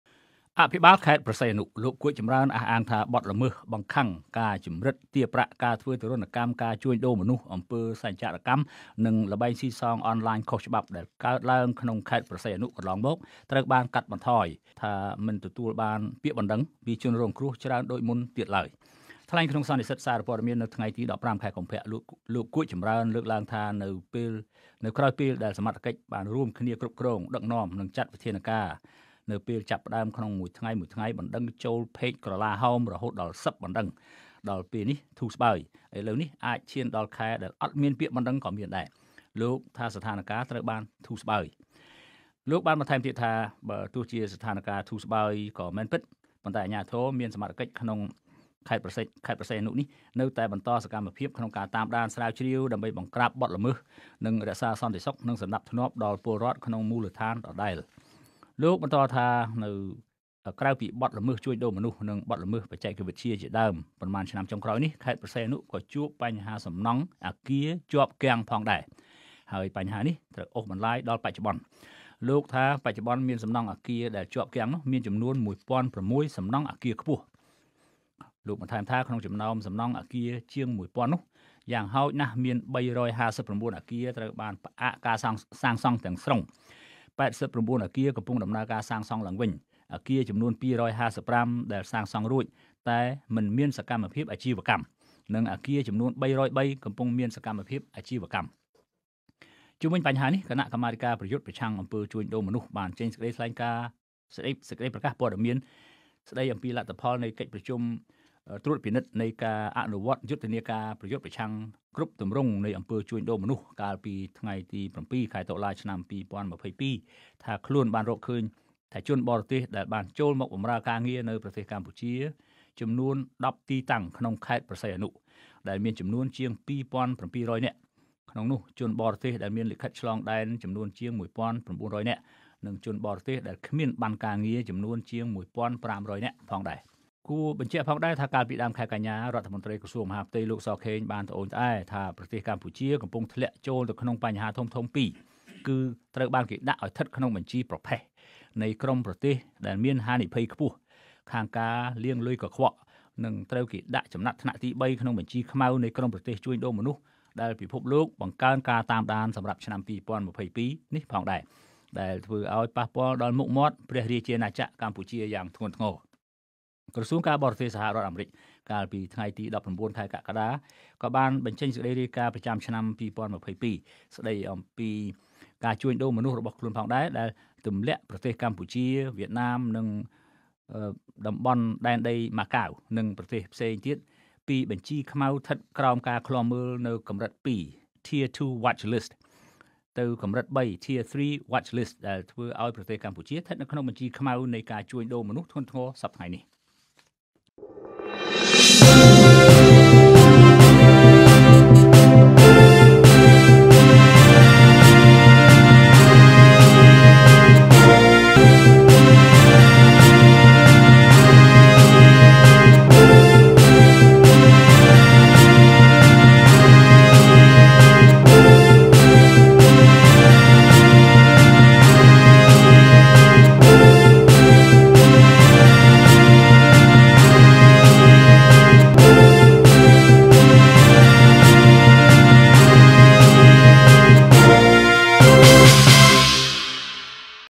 អភិបាលខេត្តព្រះសីហនុថា ការជួញដូរមនុស្សអំពើសញ្ចរកម្ម និងល្បែងស៊ីសងអនឡាញខុសច្បាប់ ថយចុះ
អភិបាលខេត្តព្រះសីហនុ លោក គួច ចំរើន អះអាងថា បទល្មើសបង្ខាំង ការជំរិតទារប្រាក់ ការធ្វើទារុណកម្ម ការជួញដូរមនុស្ស អំពើសញ្ចរកម្ម និងល្បែងស៊ីសងអនឡាញខុសច្បាប់ ដែលកើតឡើក្នុងខេត្តព្រះសីហនុ កន្លងមក ត្រូវបានកាត់បន្ថយ ព្រោះ​មិនទទួលបានពាក្យបណ្ដឹង ពីជនរងគ្រោះច្រើនដូចមុនទៀតឡើយ។